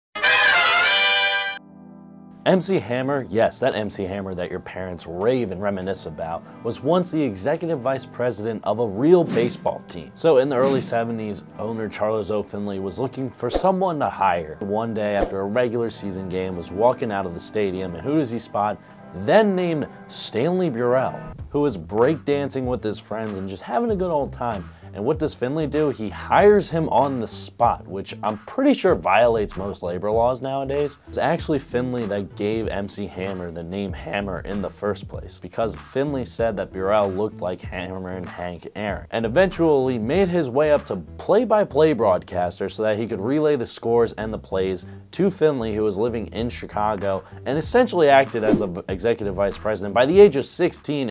MC Hammer was the Vice President of the A's | Sports History Dork
MC Hammer blew up in the 80s but before that, he was working for the Oakland Athletics as a ballboy and essentially became an executive Vice President as a teenager.

I interview entrepreneurs and cover the latest topics in sports biweekly.